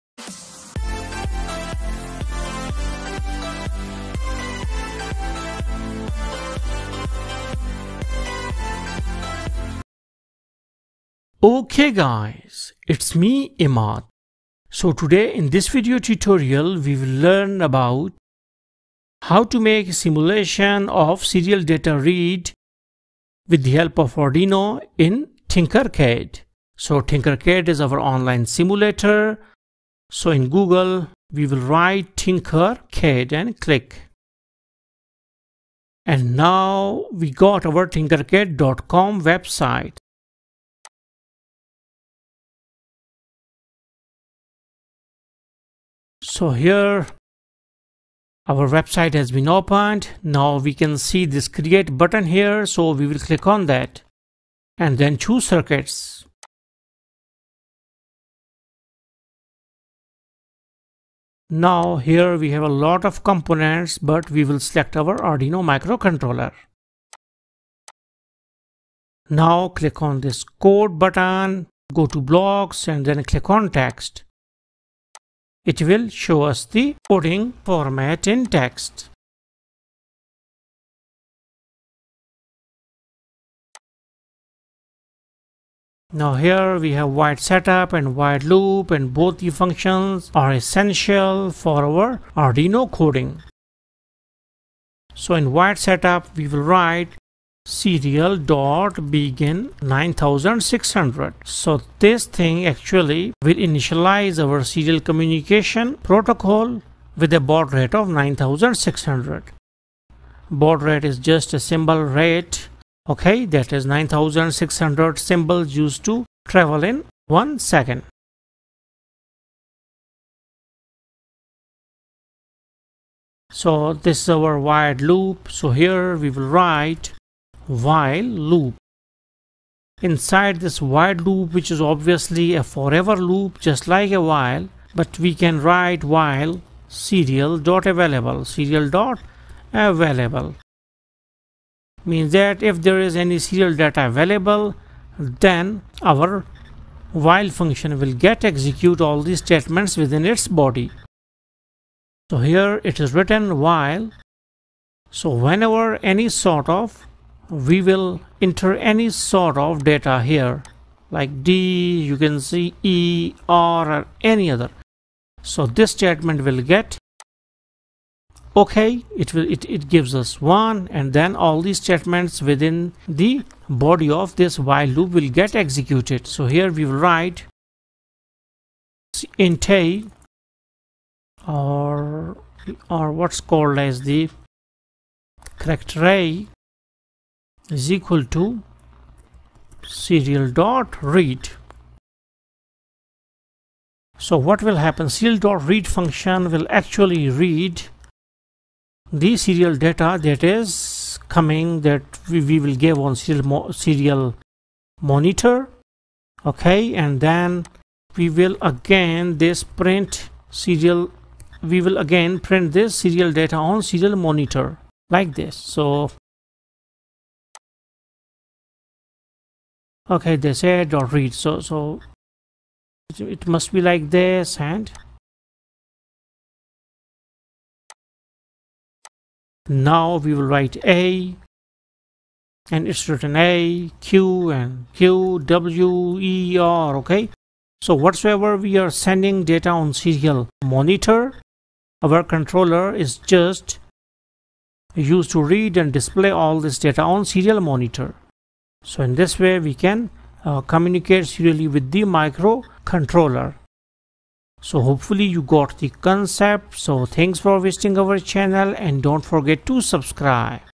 simulation of arduino serial data read in tinkercad | arduino serial communication in tinkercad
1. How to simulate arduino serial data read in tinkercad.
2. Simulation of arduino serial communication in tinkercad.
3. How to simulate arduino serial character read in tinkercad.